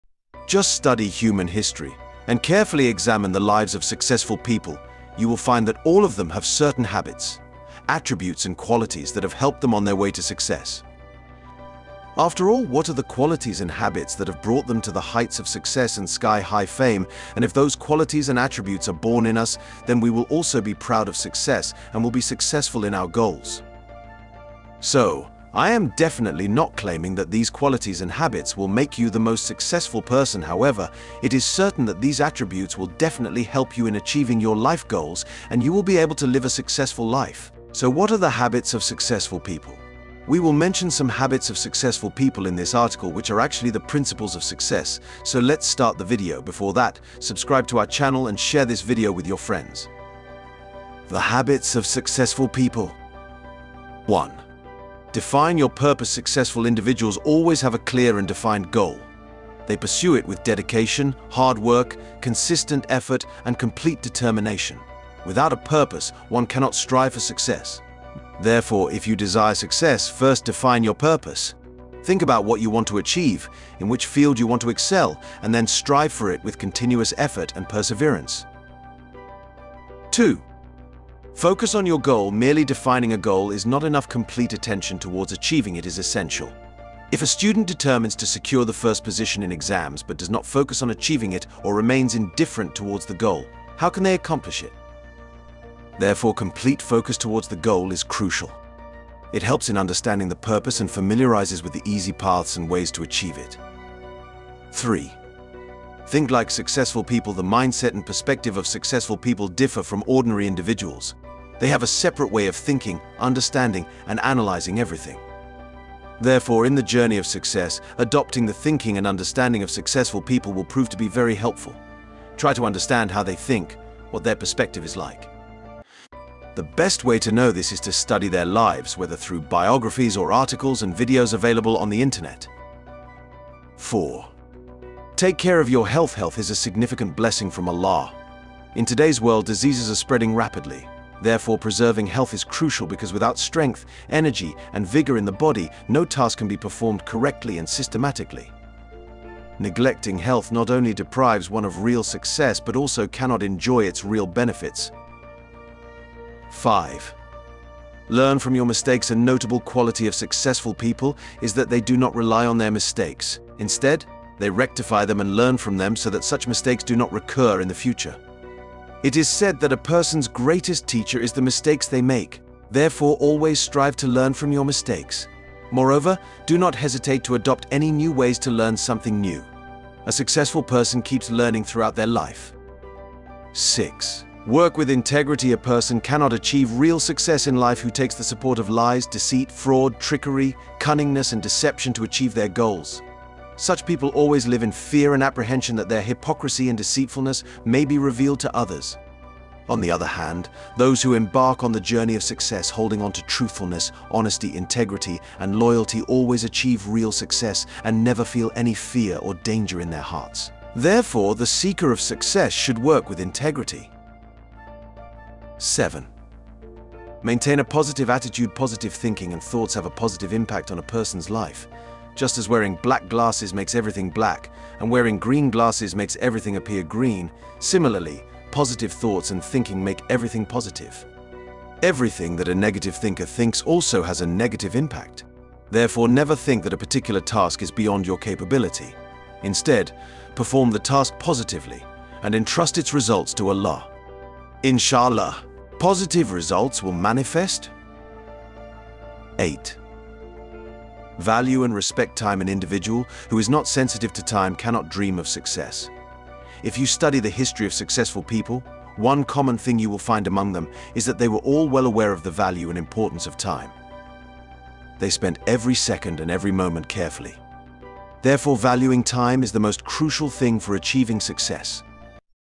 8 Habits of Successful People | Stoicism Official
8 Habits of Successful People | Stoicism Official

Just study human history and carefully examine the lives of successful people, you will find that all of them have certain habits, attributes and qualities that have helped them on their way to success. After all, what are the qualities and habits that have brought them to the heights of success and sky-high fame, and if those qualities and attributes are born in us, then we will also be proud of success and will be successful in our goals?
So, I am definitely not claiming that these qualities and habits will make you the most successful person; However, it is certain that these attributes will definitely help you in achieving your life goals, and you will be able to live a successful life. So what are the habits of successful people? We will mention some habits of successful people in this article which are actually the principles of success;